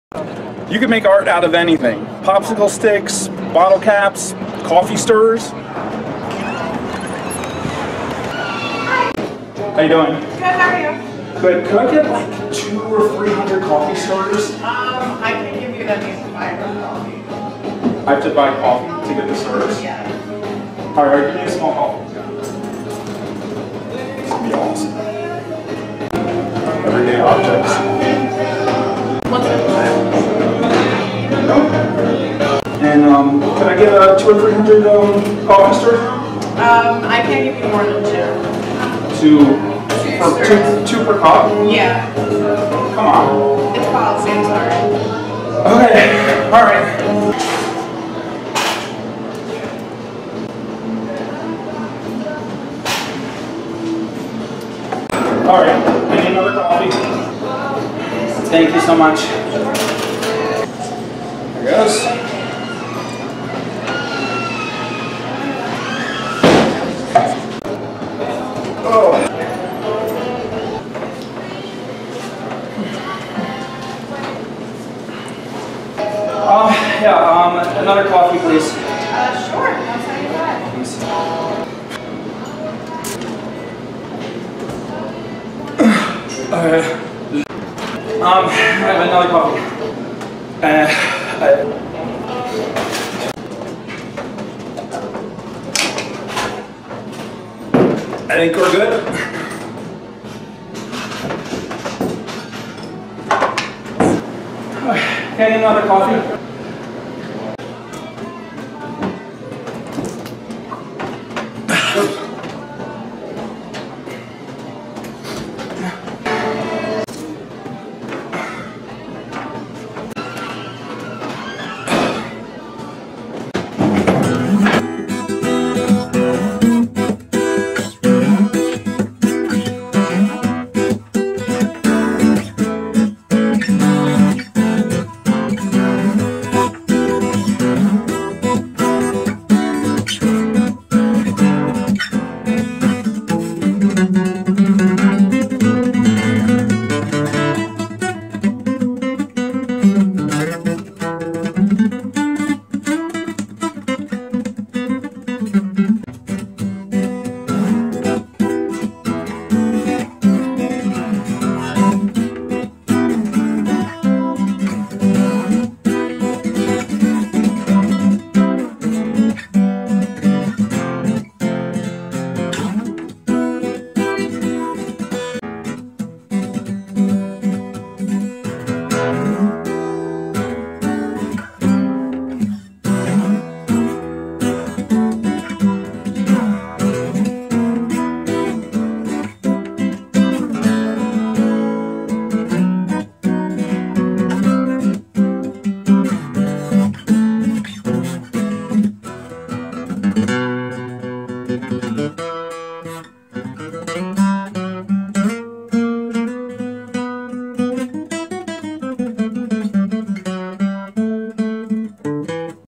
Coffee Stirrer Painting - Eps 52
A bizarre artist attempts to acquire enough coffee stirrers to make a painting.

Like for more crazy art